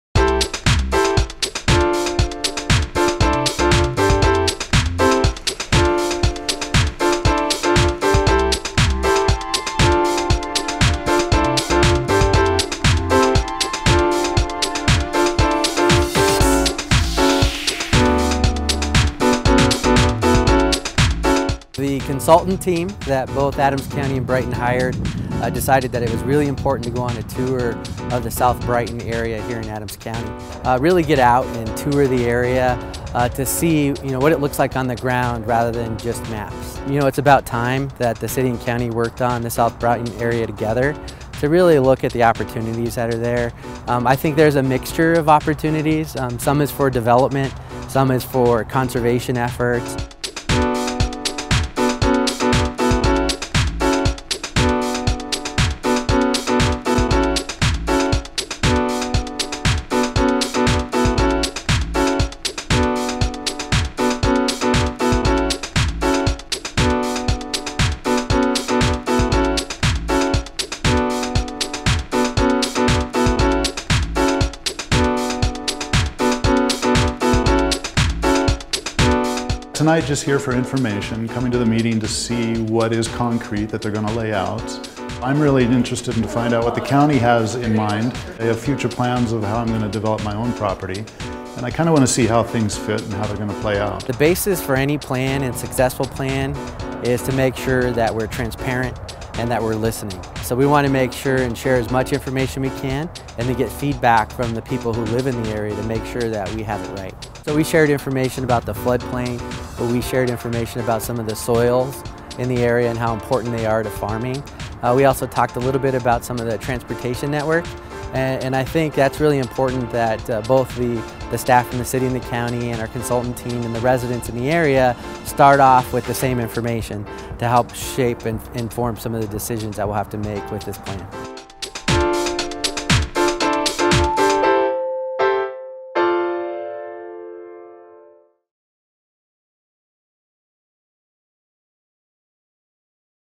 What's the Latest with the District Plan?
Adams County continues its work with the City of Brighton and others to develop a comprehensive District Plan for development in the area of South Brighton and unincorporated Adams County.  Here is a video highlighting the latest in their efforts to work with residents and officials.